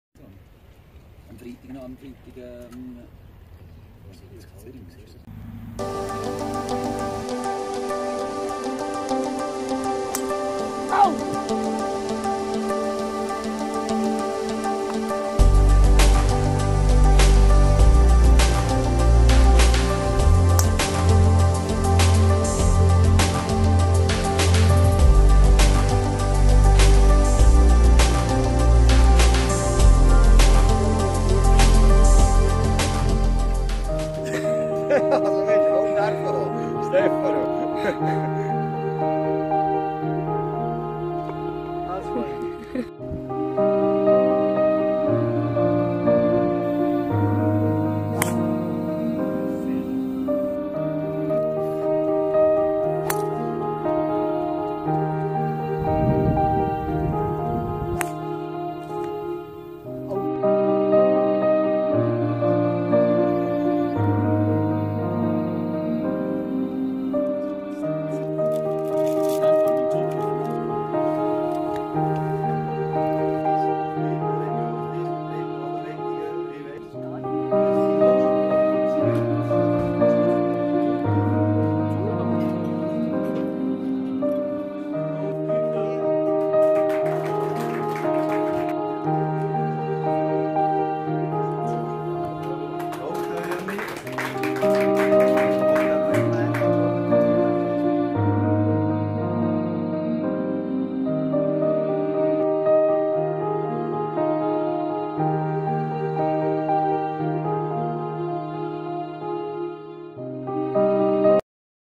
KMU SWISS Golf Leuk Impressionen
KMU SWISS Golftrophy Ausscheidungsturnier Leuk vom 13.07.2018 - Impressionen vom Golftag im Wallis